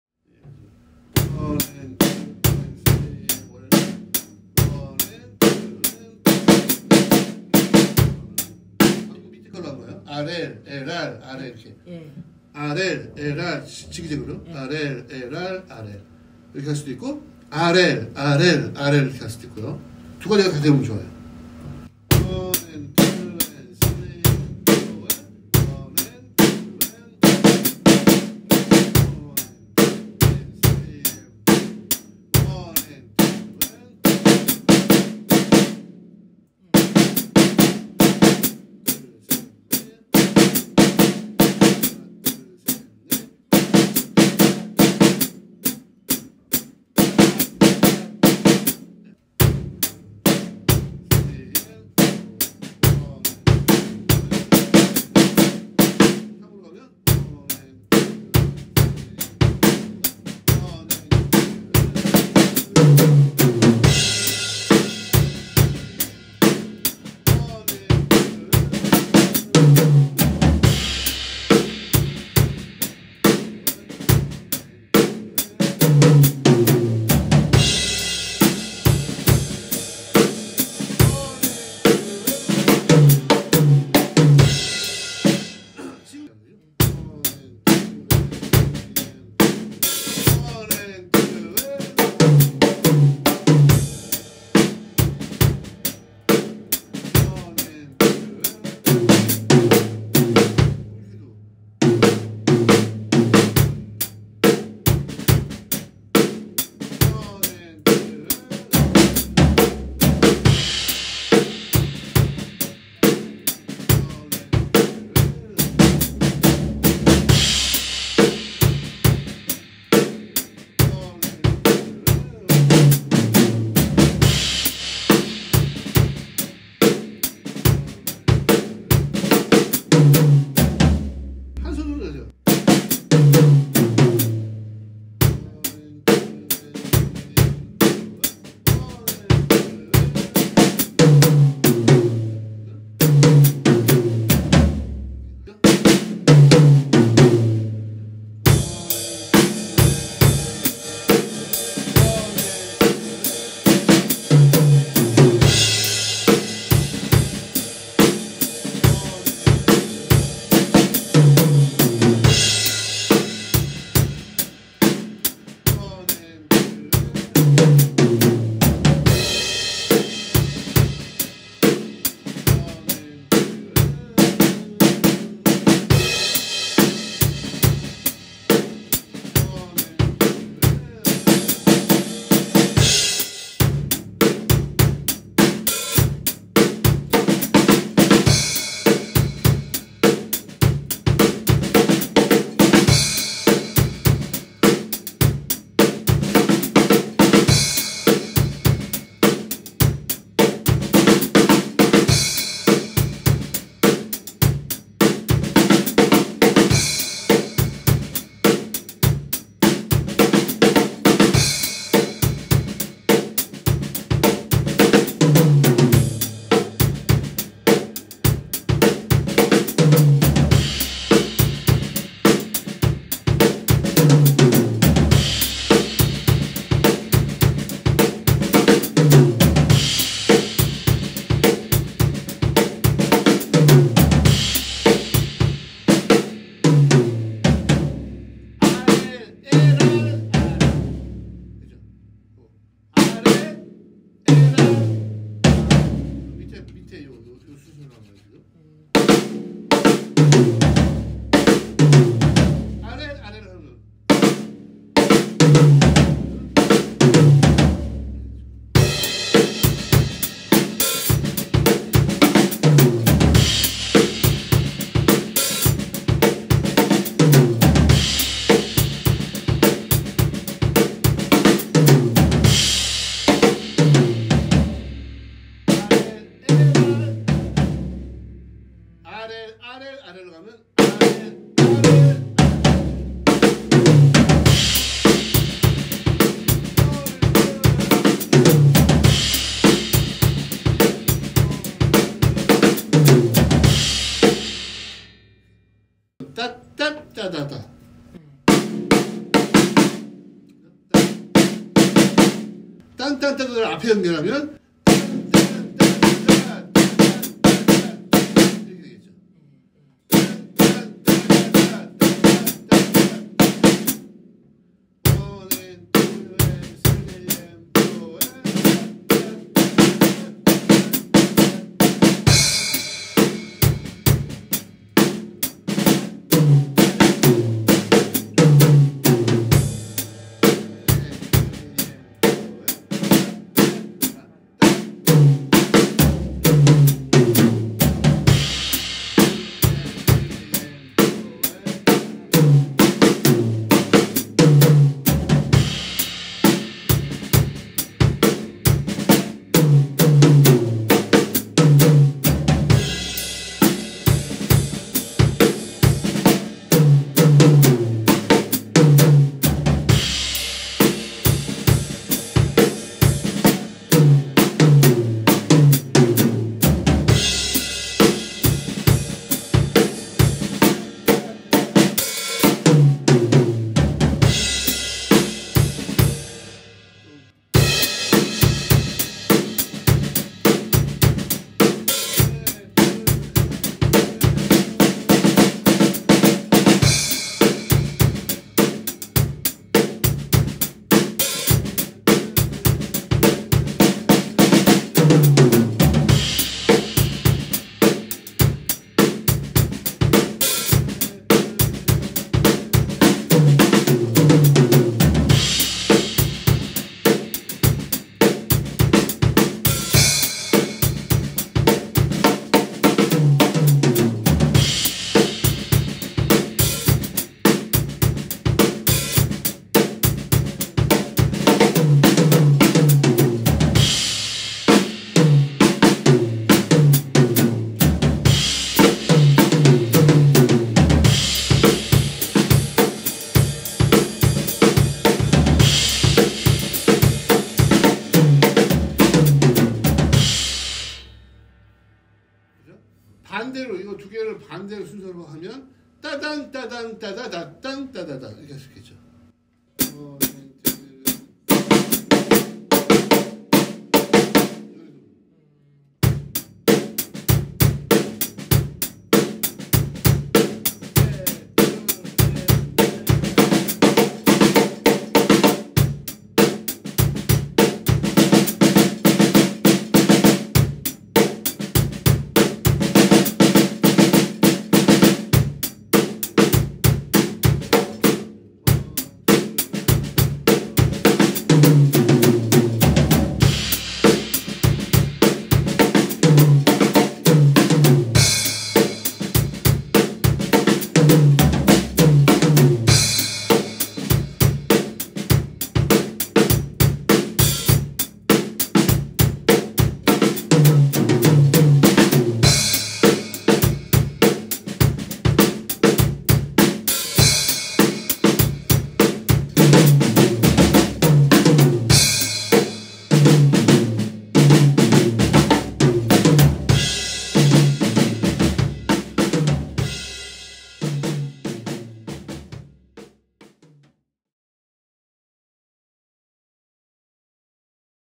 드럼레슨 옆보기12 - 짭잘한 드럼필인 만들기/ 악보제공 /드럼레슨 옆에서 보기 / 사용할만한 드럼필인
드럼레슨을 옆에서 보기~~^^

쉽게 따라할 수 있는 필인입니다~
먼저 귀로 듣고 입으로 소리를 내보세요~
그리고 손으로 필인을 만들어보세요~